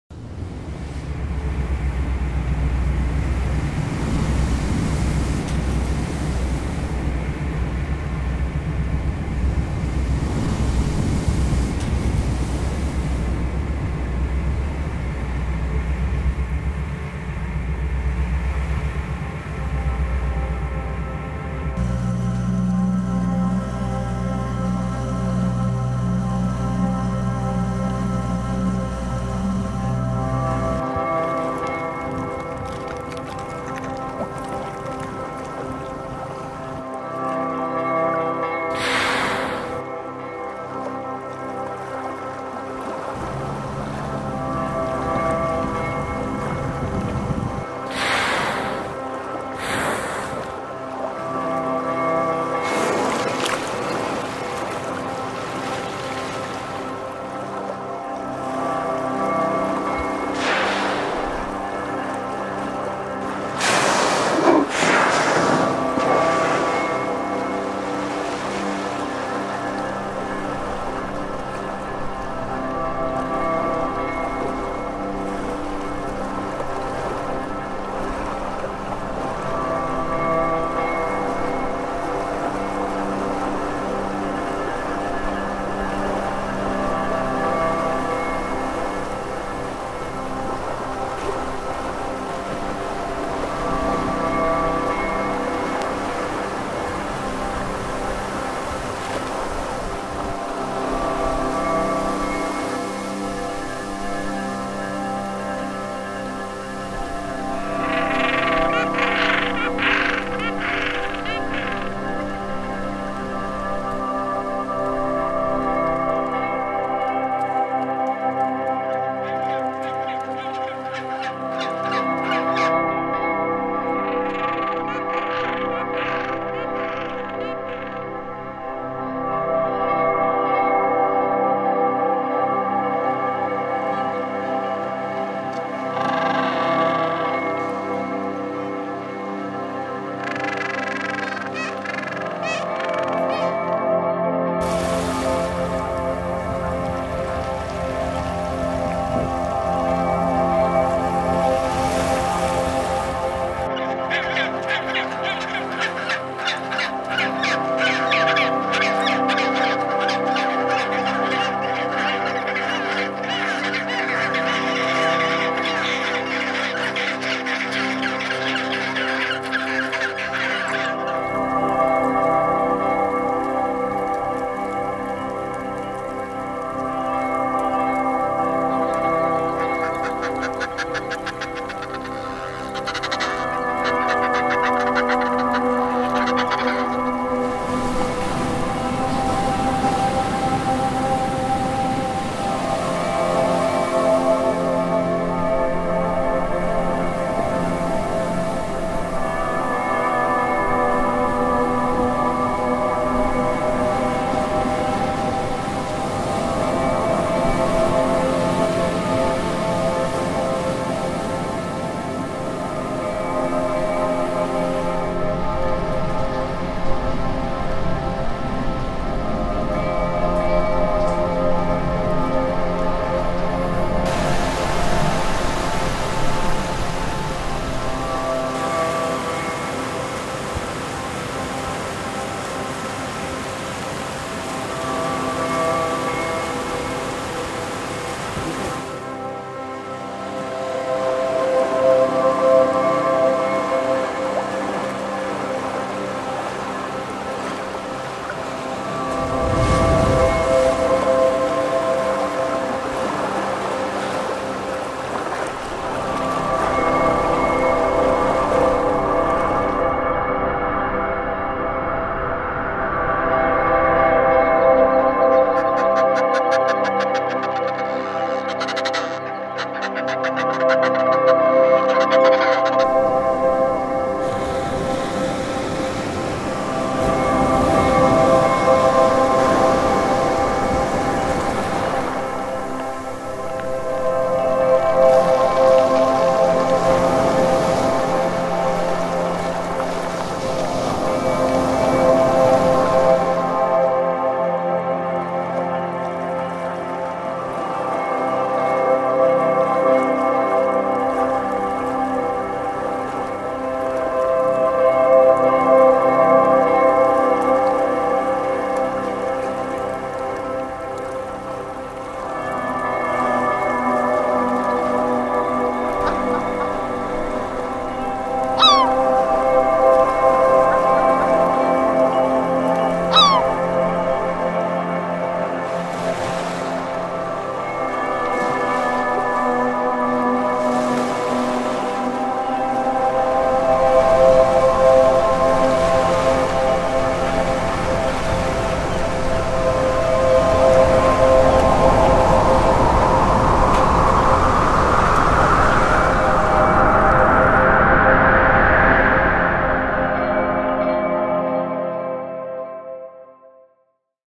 Arctic beauty
A video showcasing the beauty of the Arctic regions captured during my expeditions and photo tours to Antarctica, Iceland and Lofoten.

Video material contribution by Jeffrey Van Daele